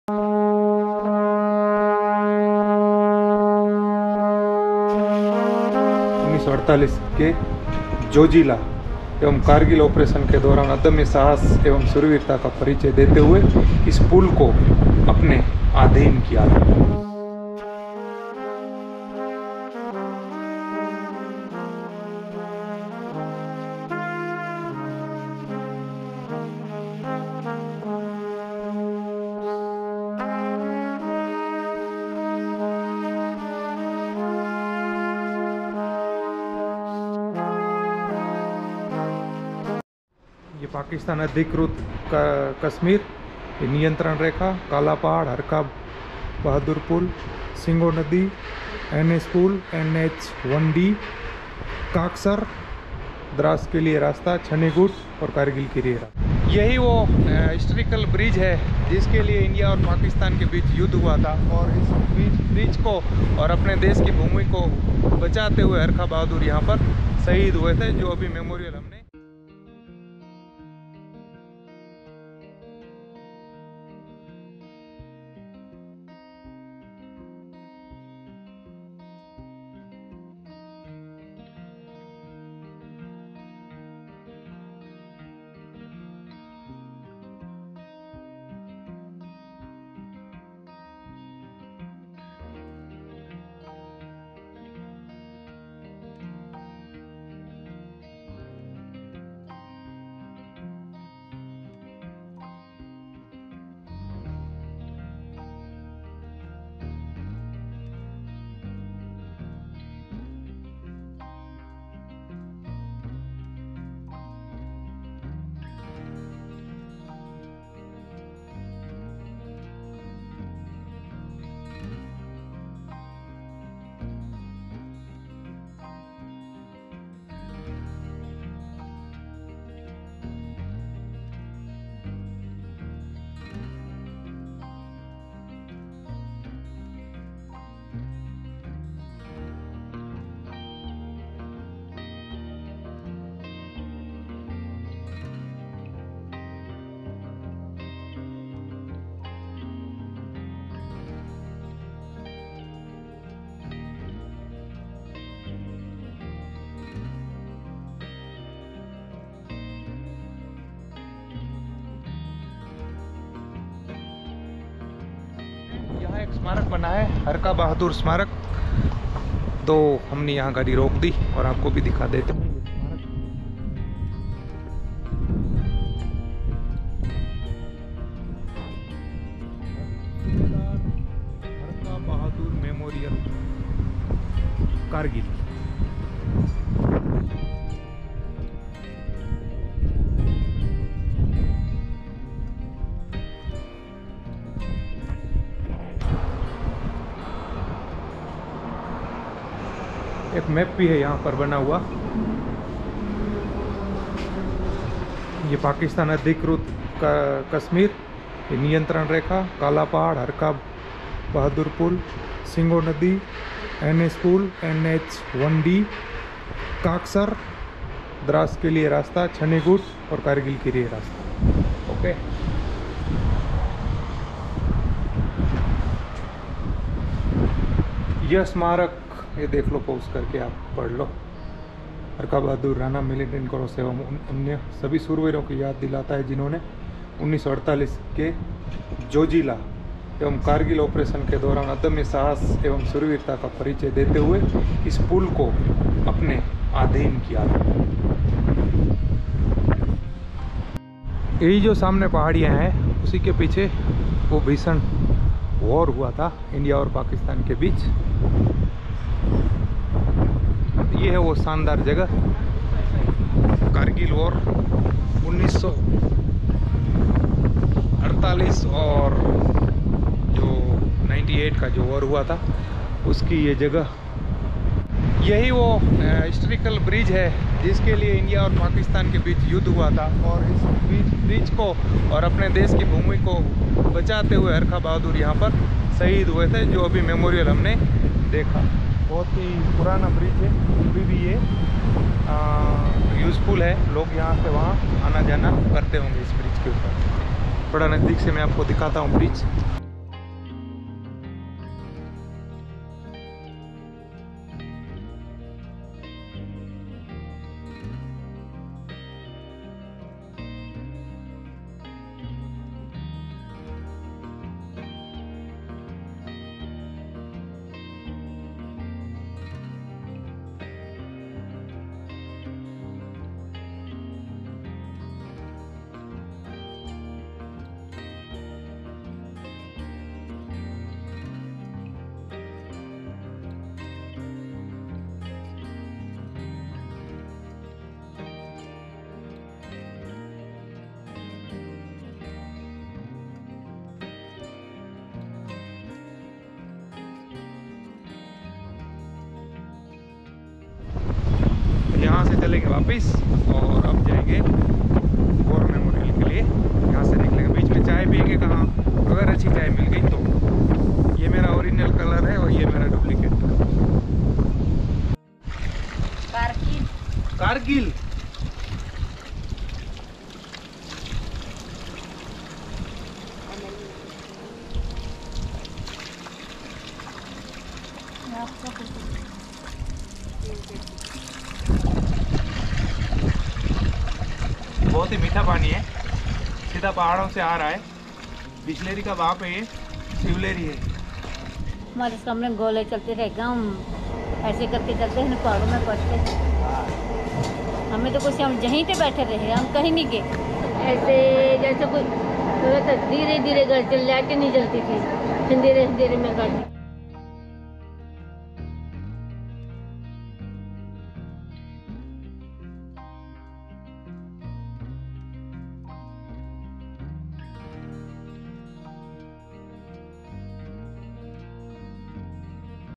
Ep.15 | यहां पाकिस्तान को युद्धमें पीछे हटना पड़ा | Prabhu the Explorer
1969 मै हुवे  भारत पाकिस्तान के युद्धमे इस पुल पर भीषण लड़ाई हुई थी अगर उस समय भारत की यहां हार होती तो सायद कारगिल आज भारत का हिस्सा नहीं होता ।

लेकिन सूबेदार हरखा बहादुर के मजबूत इरादों ने दुश्मन के दांत खट्टे कर दिए और दुश्मन को मुंह की खानी पड़ी और यह पुल आज भी भारत की विरासत बना खड़ा है ।

जय हिंद